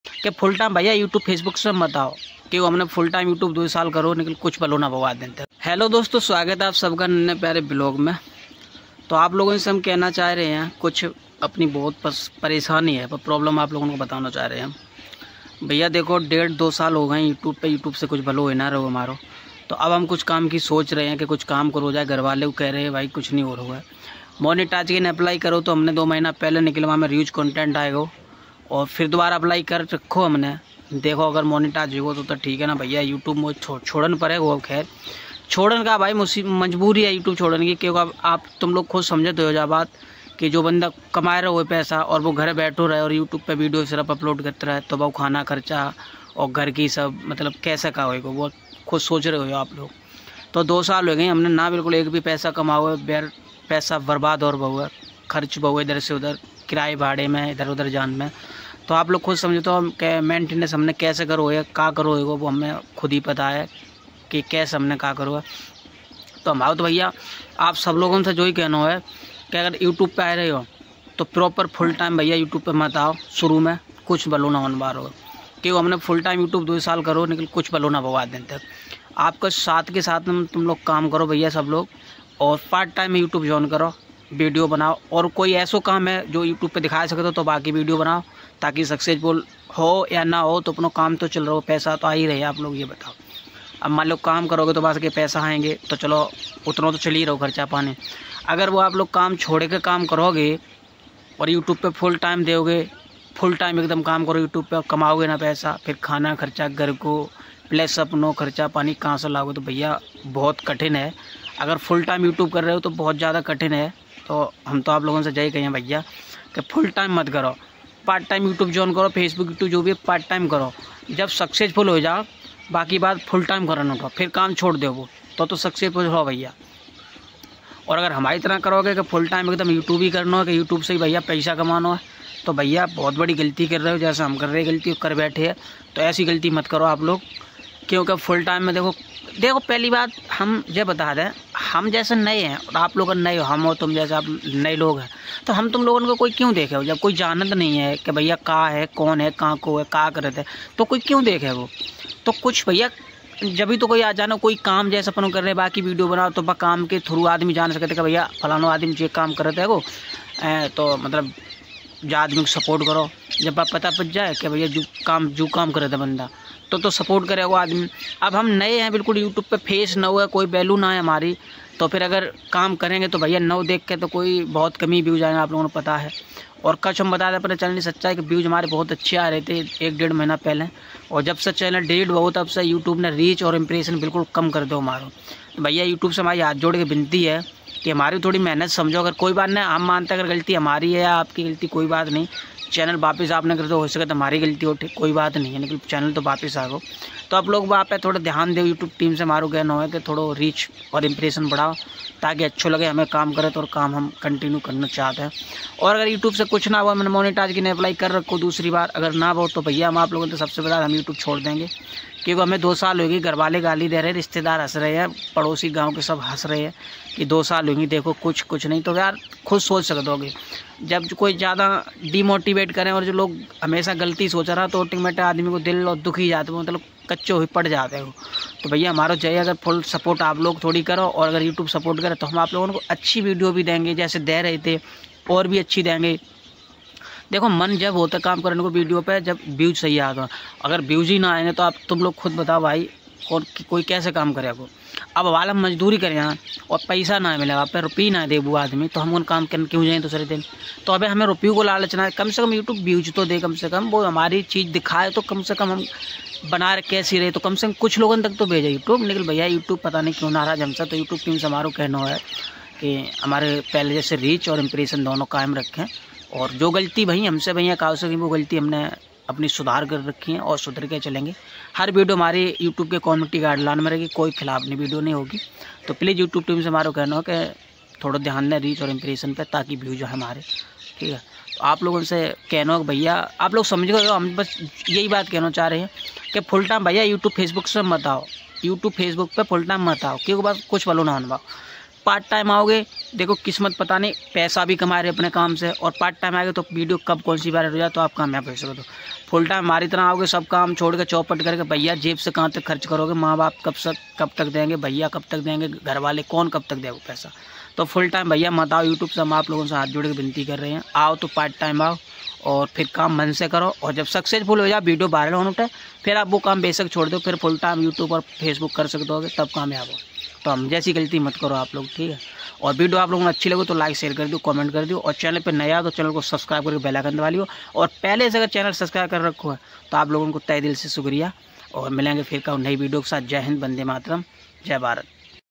फुल टाइम YouTube fecbook पर ना आओ बरना हमारी तरह बरबाद हो जाओगे 😭😭nannepyarevlogs
Youtubeparvediokesebanaye
Youtubeparfulltimekamkesekare